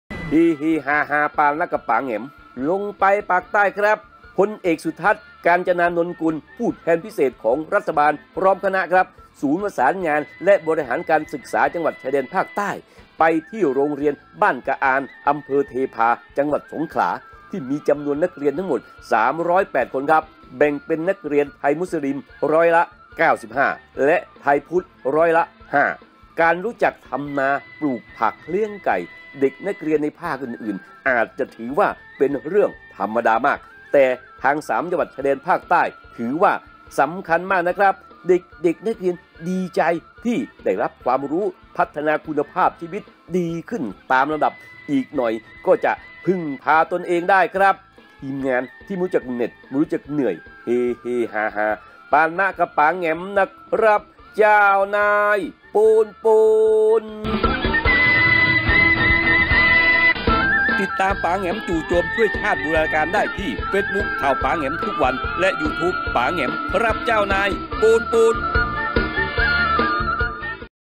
เฮๆฮาๆปานะกับป๋าแหง็ม,ผู้แทนพิเศษรัฐบาล รร กระอาน อ.เทพา
(เฮๆฮาๆ ปานะกับป๋าแหง็ม)
 ลงไปปักษ์ใต้-ครับ
 พลเอกสุทัศน์ กาญจนานนท์กุล ผู้แทนพิเศษของรัฐบาล พร้อมคณะ-ศูนย์ประสานงานและบริหารการศึกษาจังหวัดชายแดนภาคใต้..ไปที่โรงเรียนบ้านกระอาน อำเภอเทพา จังหวัดสงขลา..ที่นี่มีจำนวนนักเรียนทั้งหมด 308 คน แบ่งเป็นนักเรียนไทยมุสลิม ร้อยละ 95 และไทยพุทธร้อยละ 5
 การรู้จักทำนา ปลูกผัก เลี้ยงไก่ เด็กนักเรียนในภาคอื่นๆอาจจะถือว่าธรรมดามาก-แต่ทาง3จังหวัดชายแดนภาคใต้ถือว่าสำคัญมากนะครับเด็กๆนักเรียนดีใจ-ที่คุณภาพชีวิตดีขึ้นตามลำดับ..อีกหน่อยก็จะพึ่งพาตนเองได้..
 ทีมงานที่ไม่รู้จักเหน็ดไม่รู้จักเหนื่อย เฮๆฮาๆปานะกับป๋าแหง็ม นะครับเจ้านาย ปู๊นๆ